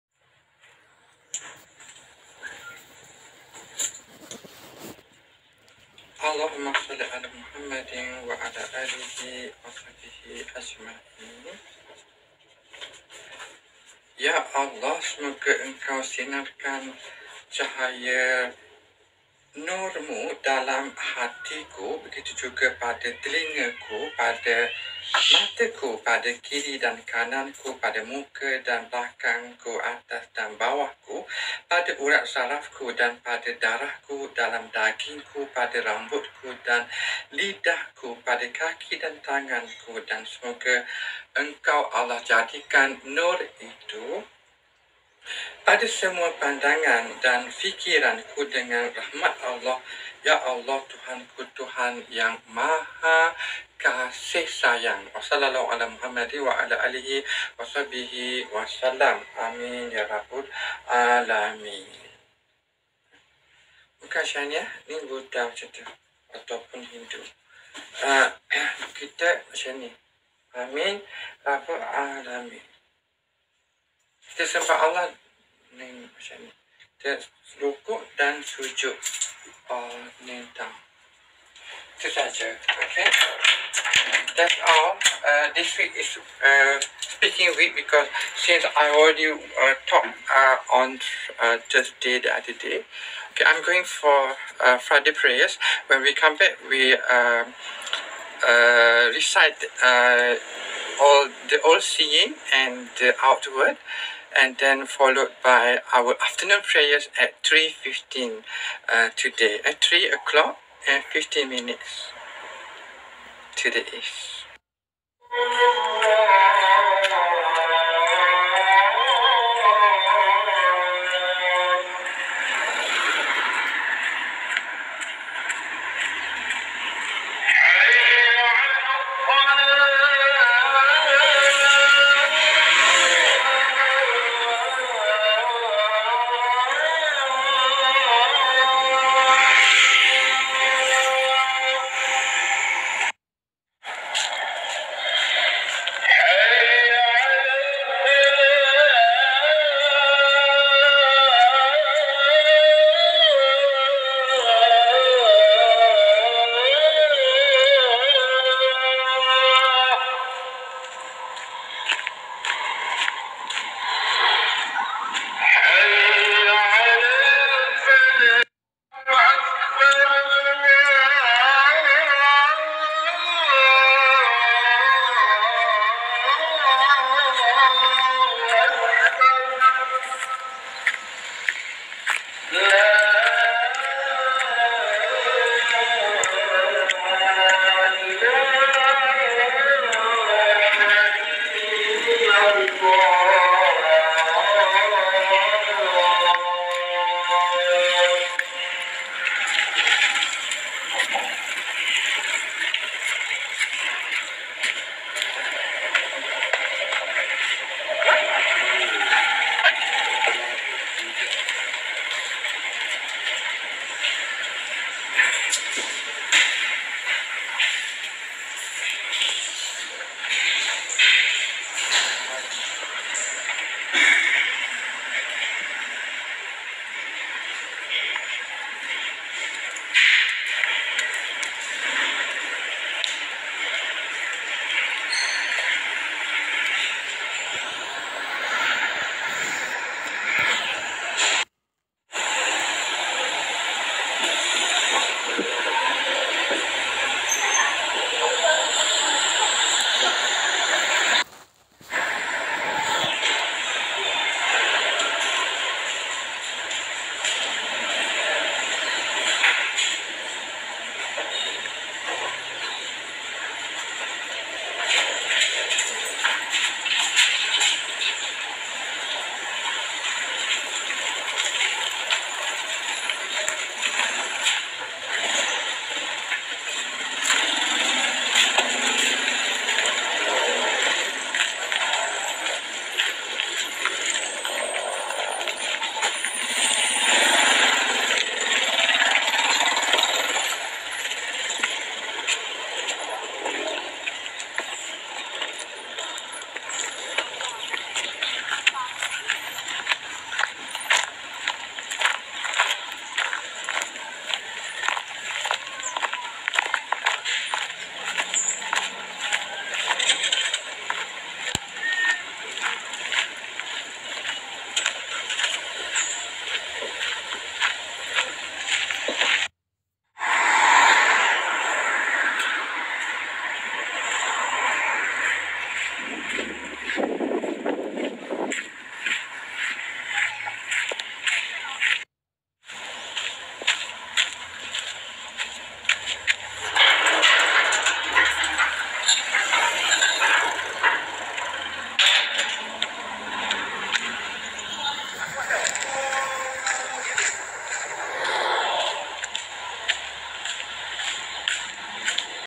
Holy Friday September 20, 2024 Before leaving Brothers Office for Friday Prayers & After 🕌🛋️
Zikrullah after Friday Prayers is Allahumma Ya Basiir;Ya Allah The All Seeing 💯 X
Allahumma Ya Zahiir 15 X;Ya Allah The Outward 15 X
Followed by Selawat upon Prophet Muhammad pbuh,his family together with us all & his friends continuesly through out of the Whole Day of Holy Friday
To recites The Holy Quran; Chapter Al-Kahfii;The Cave;Gua
At 3.15 pm make an Afternoon Prayers of Gospel without associates Allah with anyone else at all 🕒⌚🤲